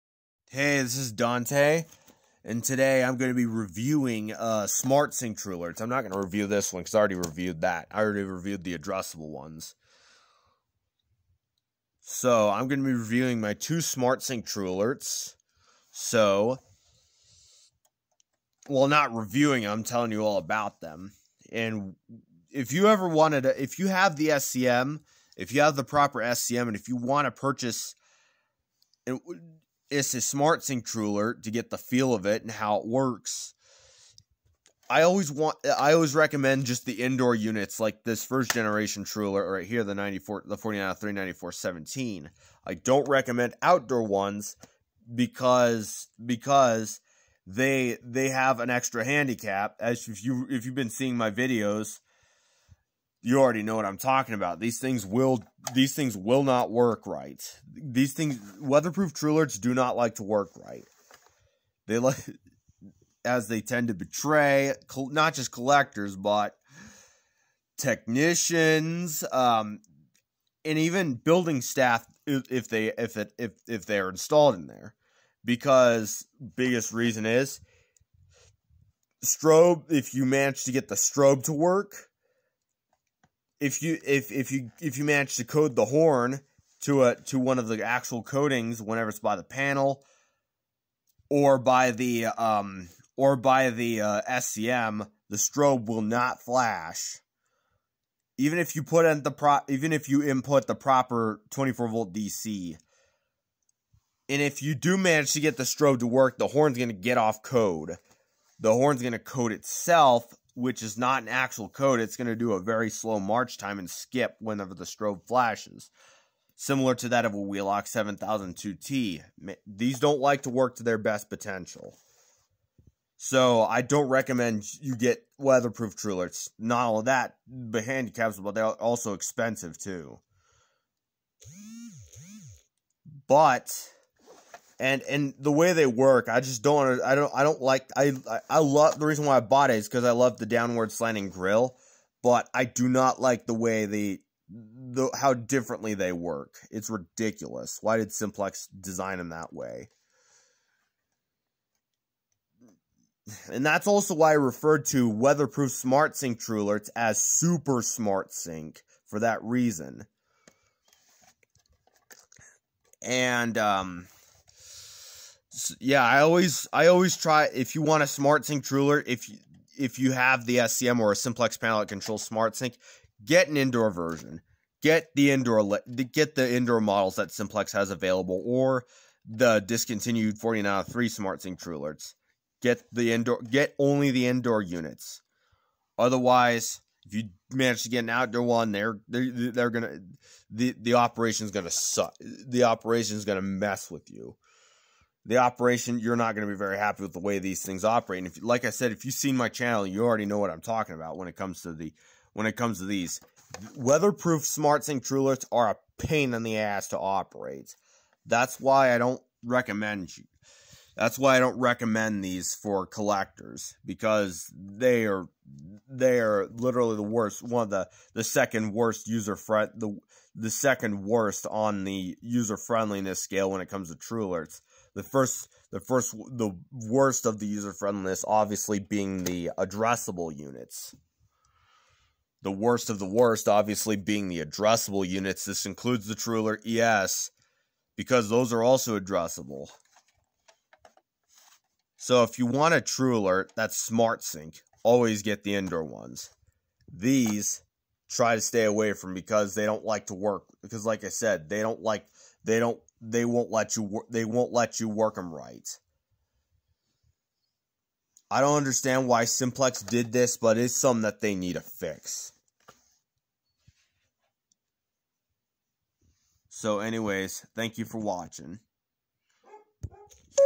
If you want a SmartSync operating Simplex TrueAlert, DO NOT BUY THE WEATHERPROOF UNITS (here's why).
I have NO idea how weatherproof TrueAlert devices pass inspection.